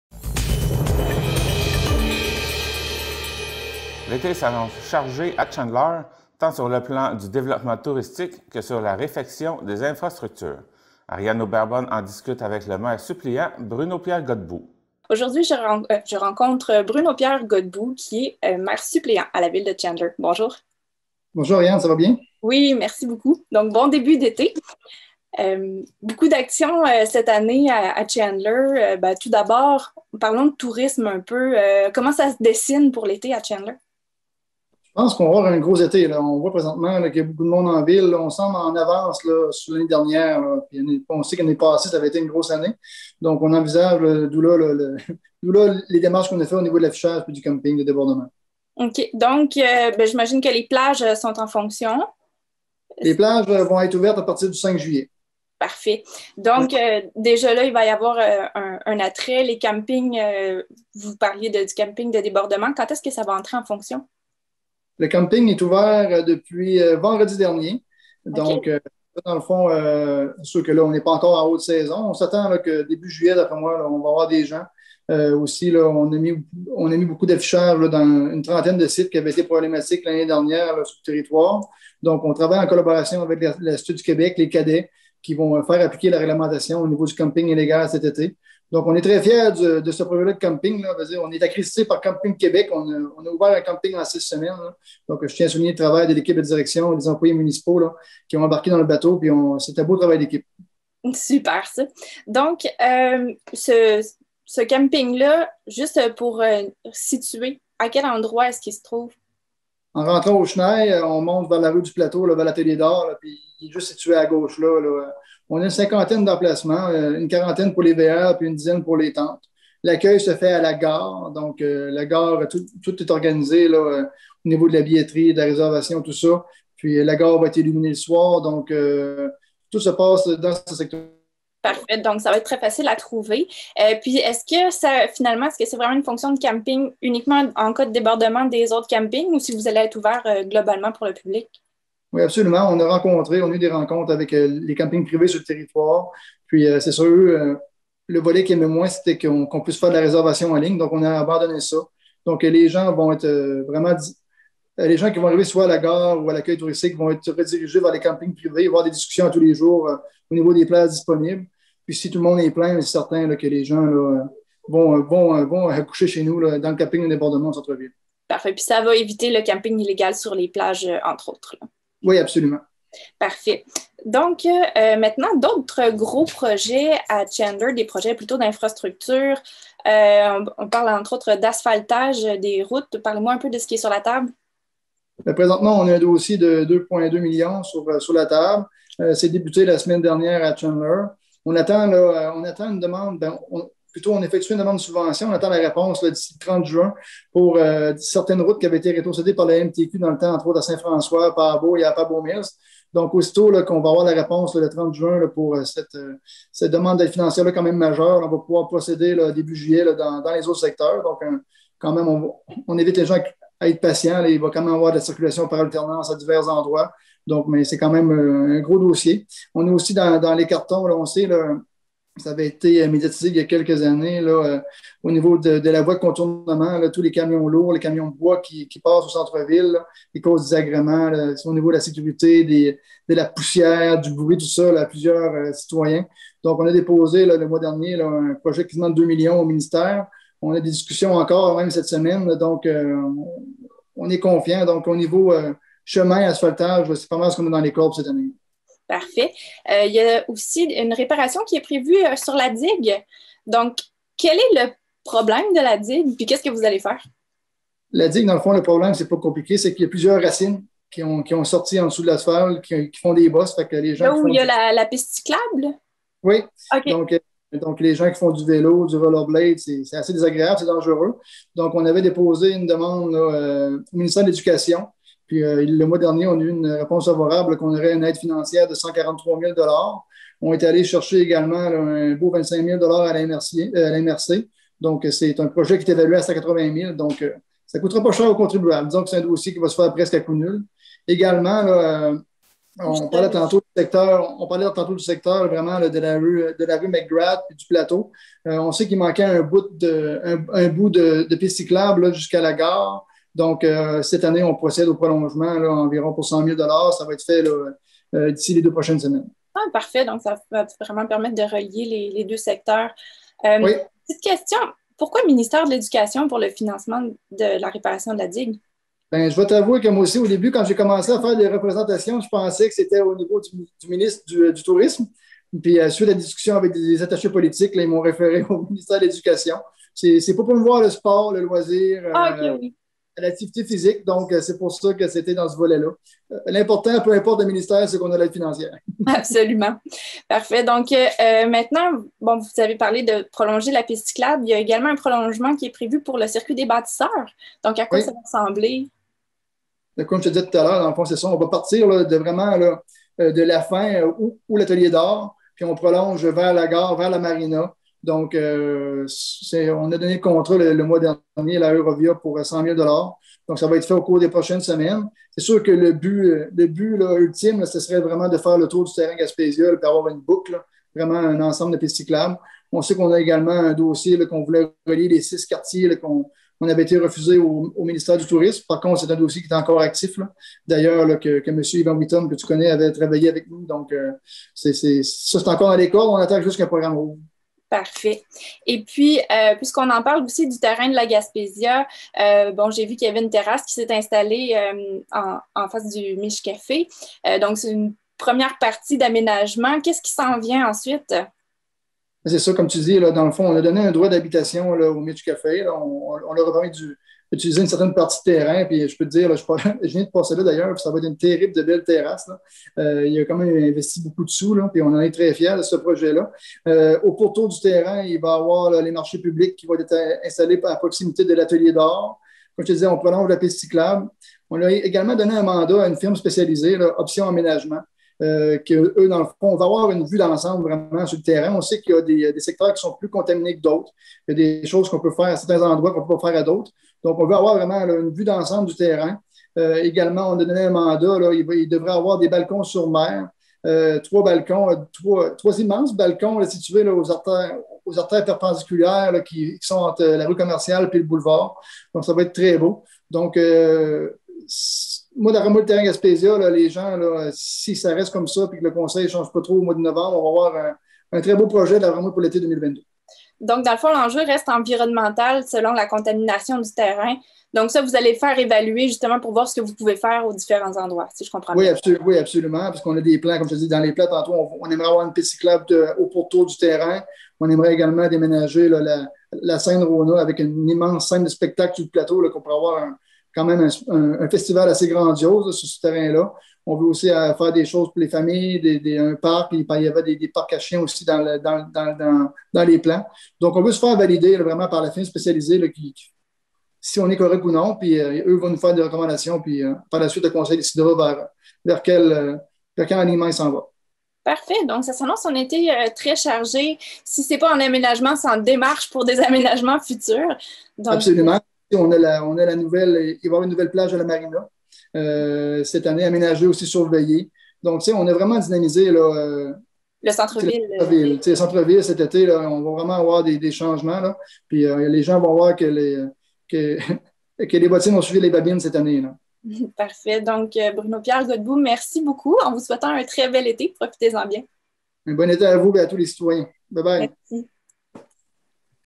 Sujets municipaux de l’heure à Chandler avec le maire suppléant Bruno Pierre Godbout
L'été s'annonce chargé à Chandler, tant sur le plan du développement touristique que sur la réfection des infrastructures. Ariane Aubert Bonn en discute avec le maire suppléant Bruno-Pierre Godbout.